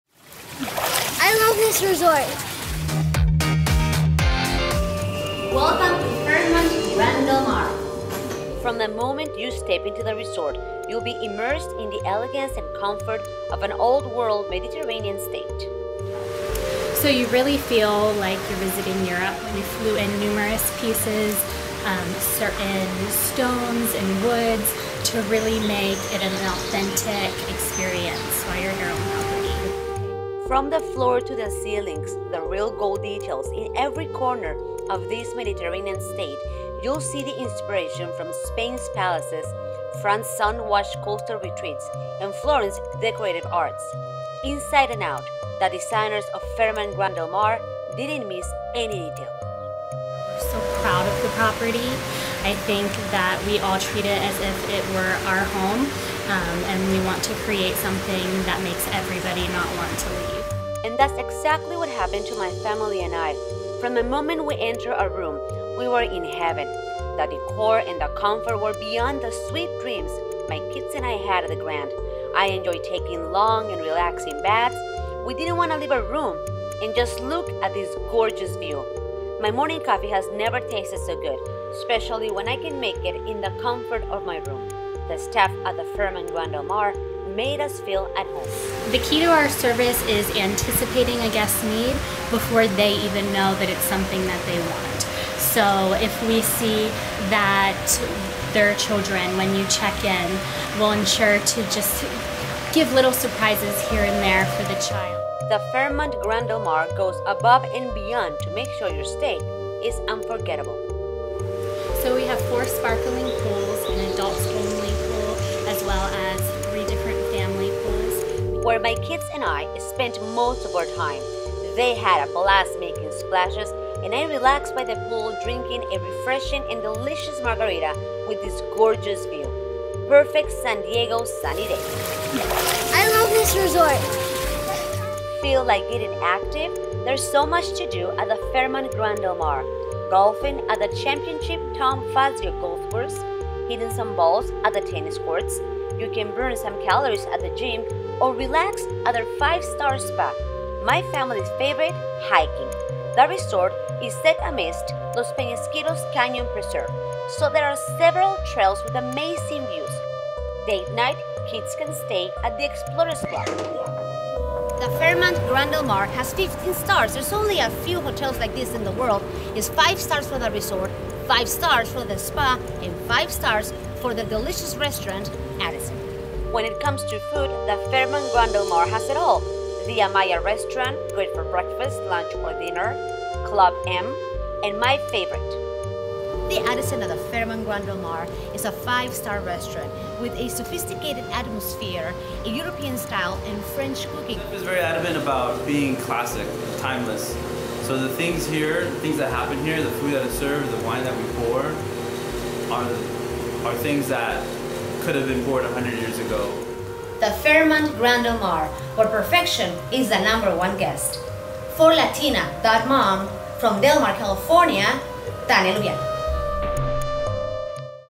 Fairmont Grand Del Mar is Luxury & Perfection (Review)
My family and I have stayed at amazing resorts but our experience at the Fairmont Grand Del Mar was like no other we've ever had. Fairmont Grand Del Mar is True Luxury & Perfection!
From the accommodations, the service, amenities to the delicious food! Every moment we spent at Fairmont Grand Del Mar, a 15 Stars Resort, was beyond our expectations.
WATCH THE VIDEO to see why Fairmont Grand Del mar in San Diego County, California is one of the best Luxury resorts in the world!!